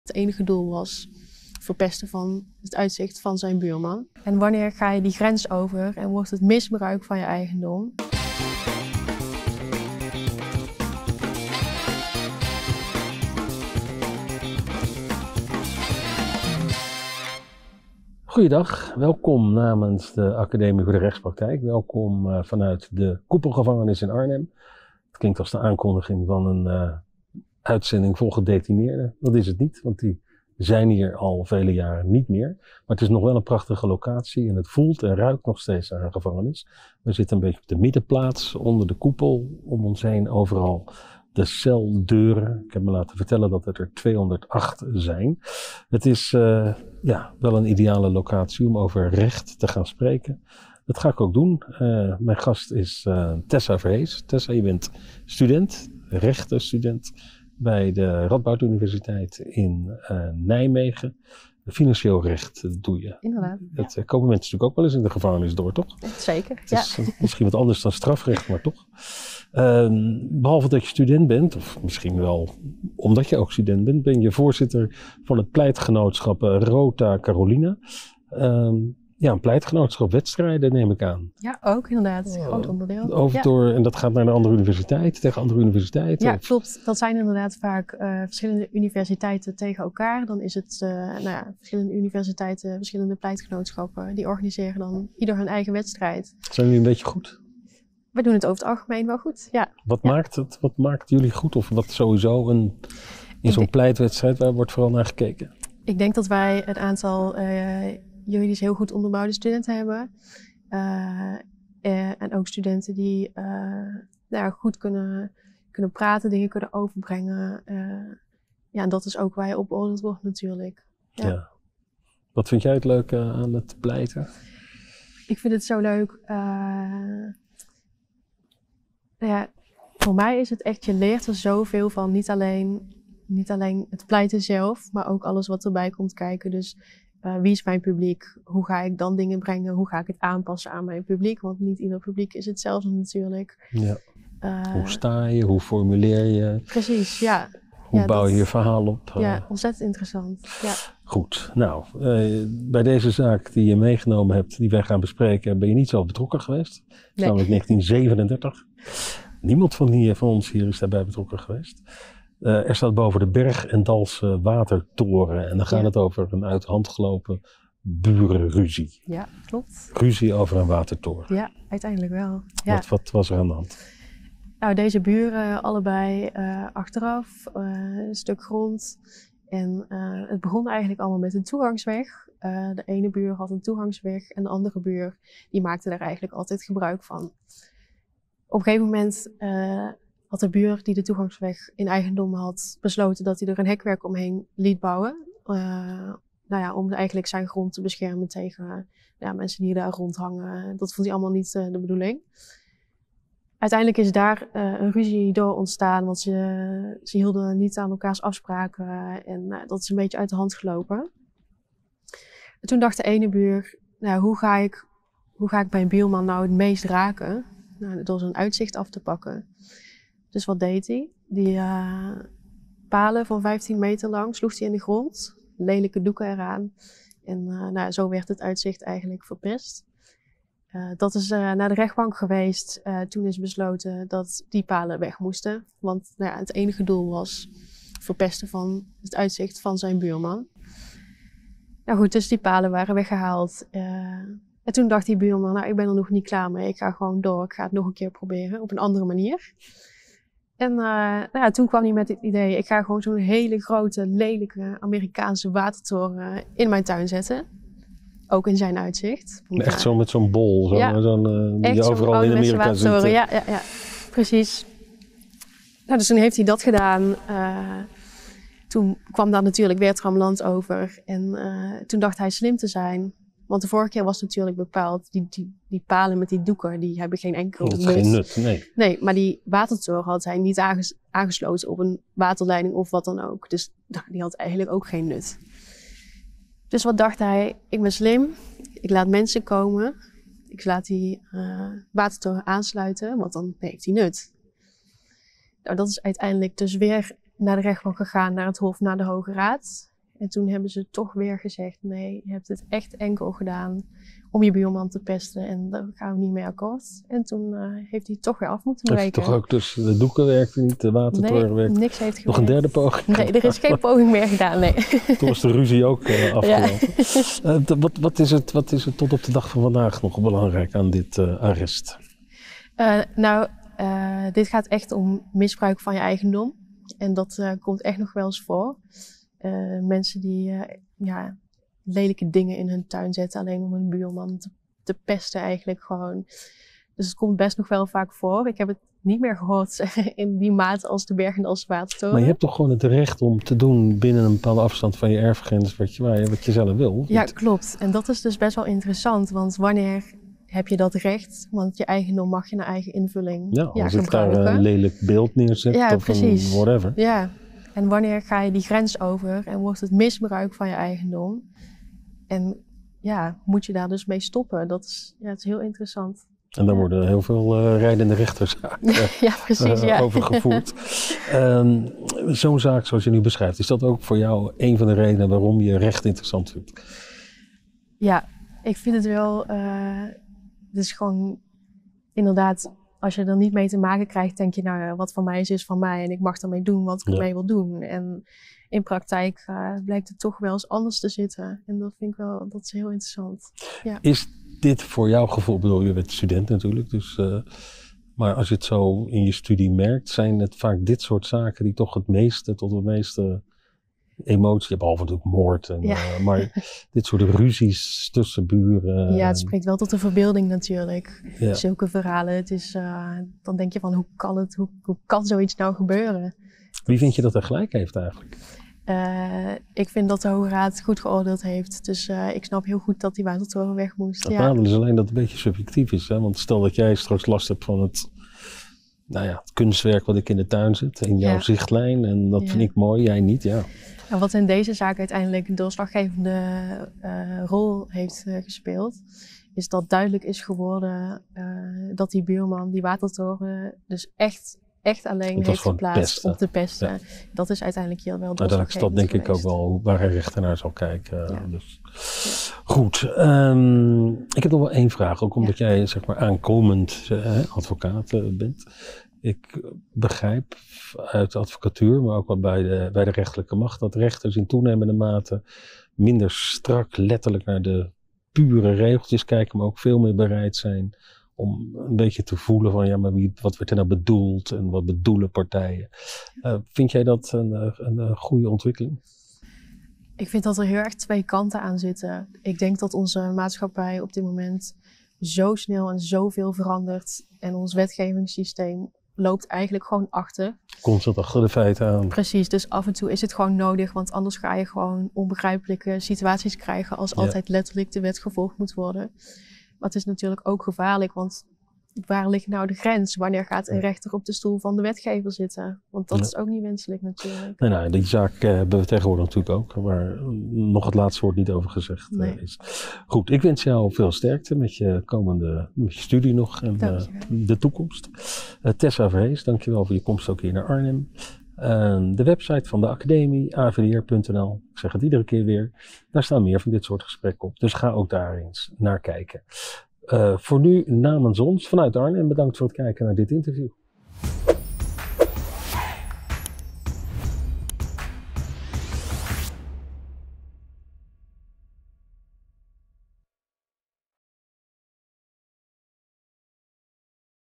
Tessa Verhees | Berg en Dalse watertoren, een uit de hand gelopen burenruzie.
Aanleiding voor deze rechtsgang was een burenruzie. Deze ruzie is begonnen om een simpele toegangsweg over het landgoed van een van de buren. Toen dit niet goed opgelost kon worden besloot een van de buren de andere buur te raken in zijn meest dierbare bezit, het uitzicht op zijn landgoed. Hij deed dit door drie palen in de grond te zetten van 15 meter hoog, met lelijke doeken wapperend aan de bovenkant. De benadeelde buur deed zijn beklag bij de rechtbank en kreeg gelijk, de palen moesten weg. Hiermee was de ruzie nog niet ten einde gekomen. Zijn buurman plaatste nu een grote watertoren, recht voor het uitzicht van de buurman. Aanvankelijk was deze watertoren niet eens aangesloten, waardoor het geen ander doel diende dan het dwarsbomen van de buurman. Later is de watertoren toch nog aangesloten, in de hoop dat de toren op die manier toch mocht blijven staan. Uiteindelijk is het anders gelopen, de toren moest weg. Dit is misbruik van een eigendomsrecht. De toren is enkel neergezet om de buurman te hinderen.

Jurisprudentie
Hoge Raad 2 april 1937, ECLI:NL:HR:1937:13